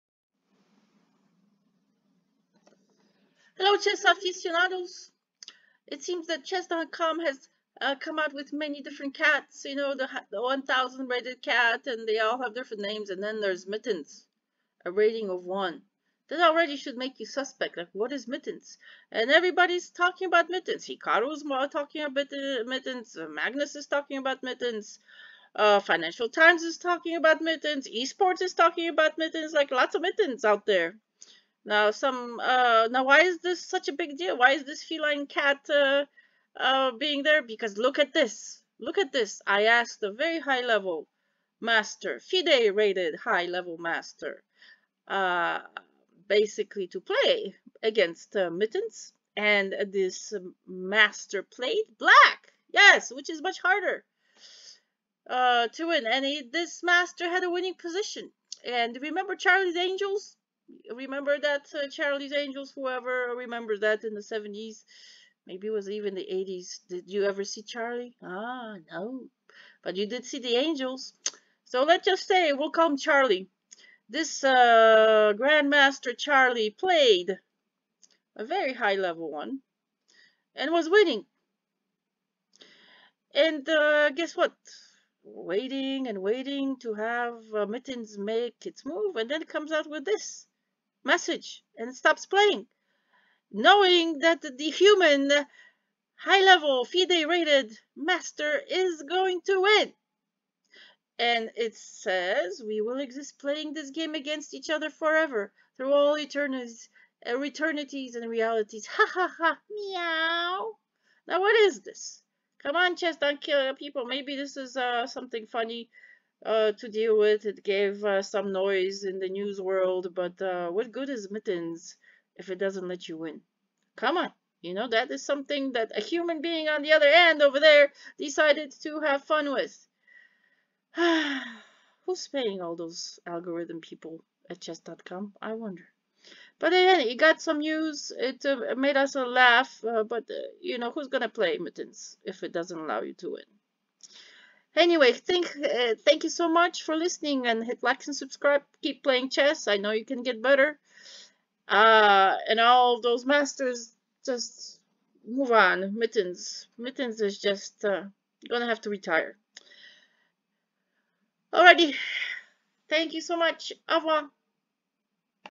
Mittens did this to a FIDE high level rated master!
The extravagant new Mittens can be found on chess.com with other felines. This feline bot is quite a doozy. I asked a very ..and I mean VERY high rated FIDE master to play a friendly game with this Mittens and you will not believe what this kitty cat did. click and find out..and he played MITEENS WITH BLACK AND WAS WINNING IN THE ENDGAME!!